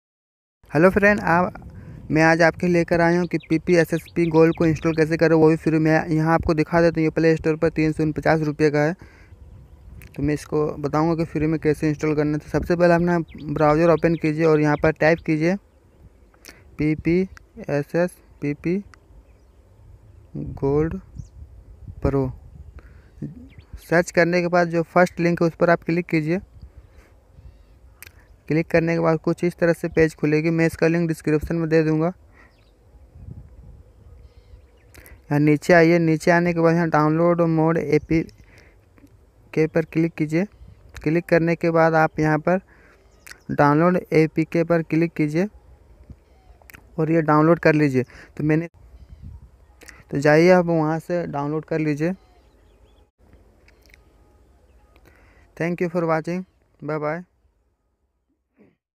How to download PPSSPP GOLD PRO Free
How to download ppsspp gold pro PSP emulator free any android mobilr